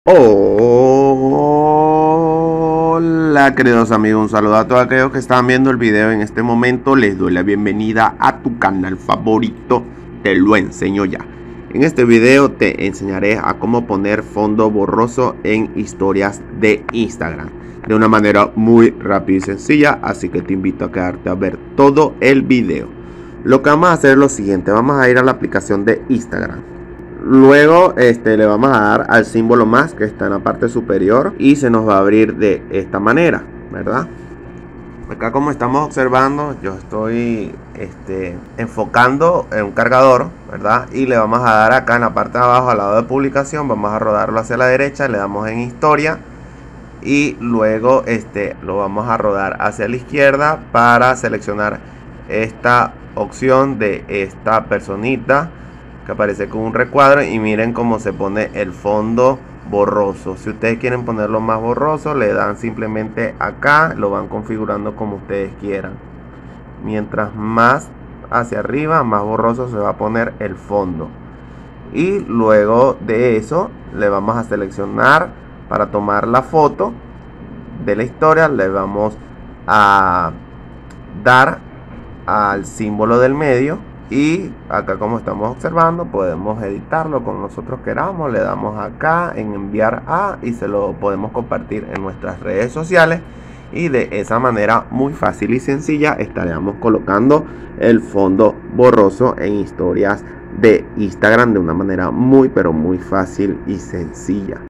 Como Poner Fondo Borroso en Historias de Instagram I Solución 2025
En este video te enseñare a Como Poner Fondo Borroso en Historias de Instagram de una manera fácil y sencilla

Videos Recomendados
💛 Como ACELERAR Celular